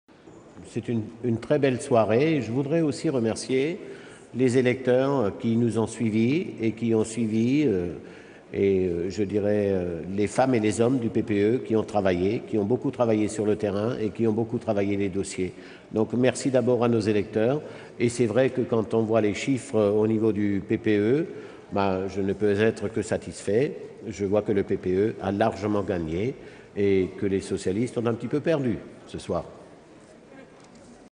Joseph Daul: EPP has "Clearly" Won 2009 European Elections
Joseph Daul, President of the European People's Party, which remained the biggest group in the European Parliament in Sunday's elections, says his group "clearly" is the winner.

EPP retained 263 seats in the parliament, or 35.7 percent, down from 288, or 35.7 percent, in the previous parliament.